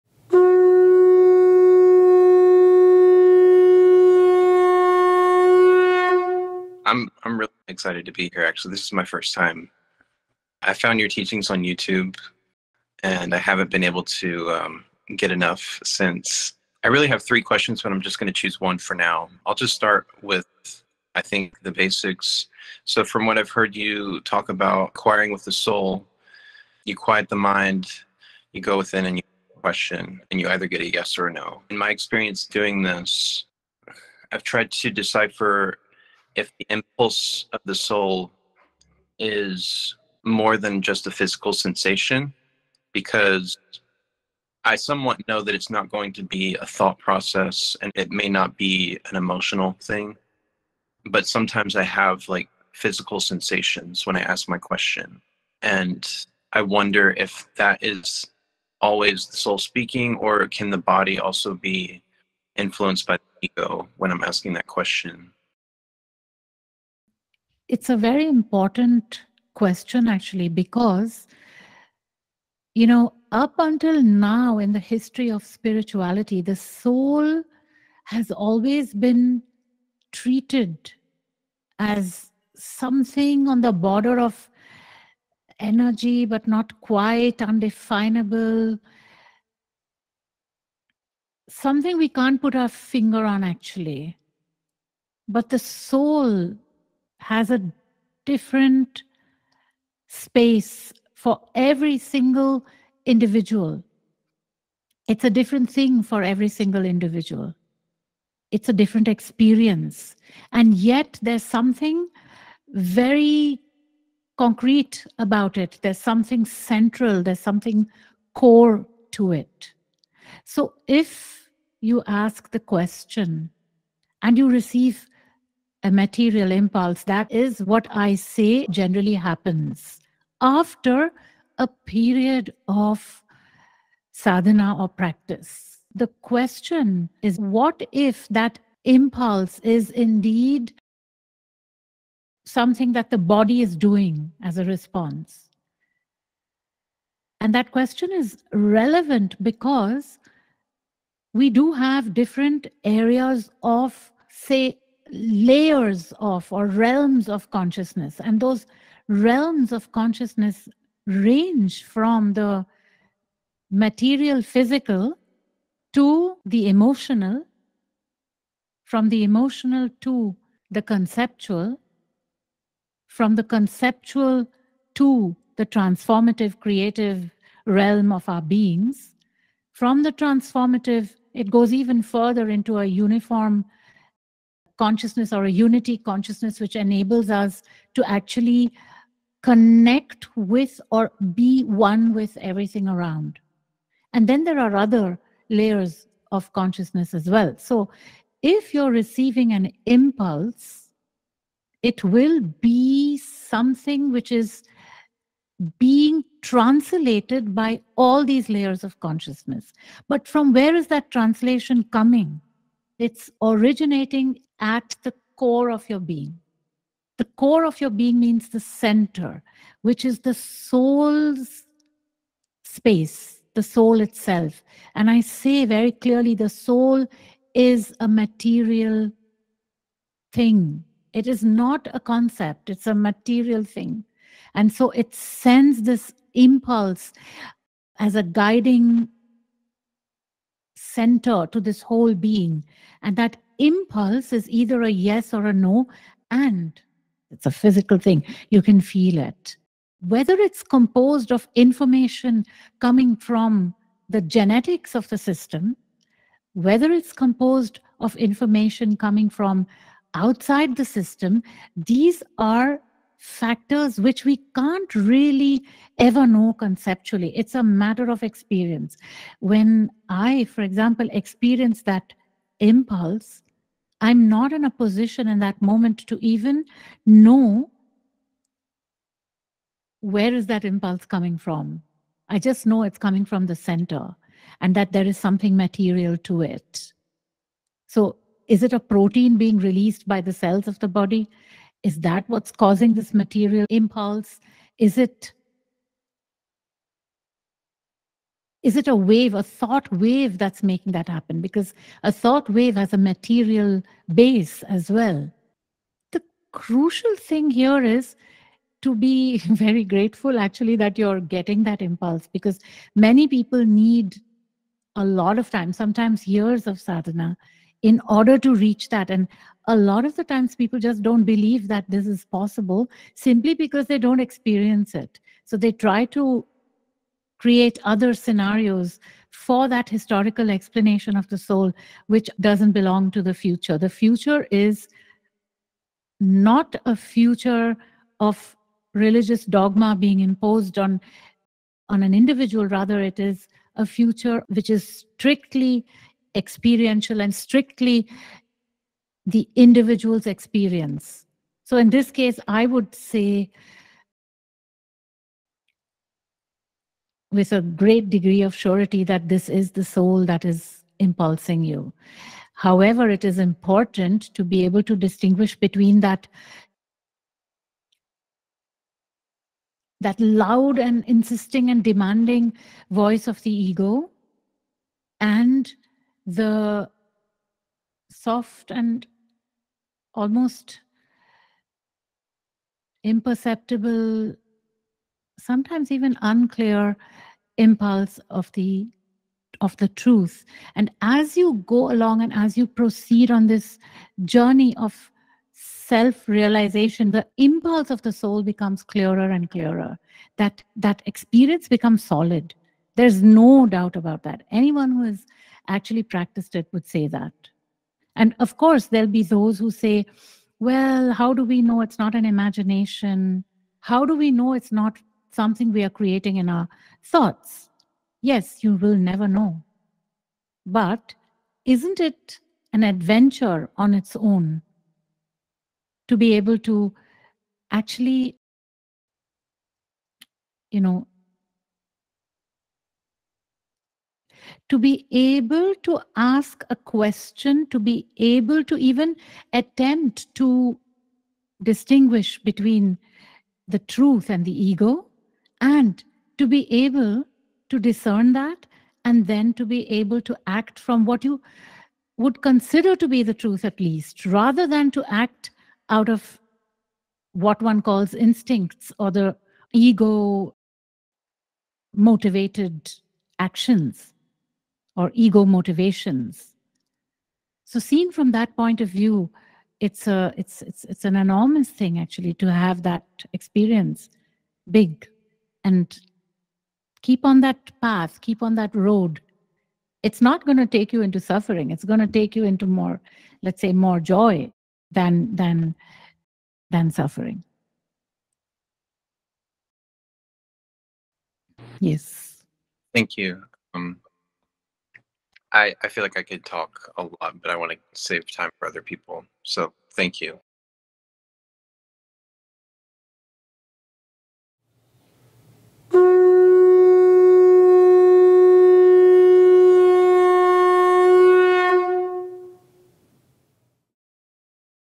Maharishikaa | I did the practice! Is this the Truth Impulse? | Preeti Upanishad
Having recently started the Maharishikaa practice of tuning inward to sense the Impulse of the Soul - Jeremiah Harmon asks the Maharishikaa about the physical sensations that he experiences when he receives an impulse - could this be the ego? Maharishikaa clears up the fog that has historically surrounded the word 'Soul'. Demystifying this ancient word, she describes how the Soul communicates with the individual, validating Jeremiah's experience, as she sheds light on the future, as humankind gradually shifts from the religious experience to the spiritual one, from religious dogma to spiritual knowledge.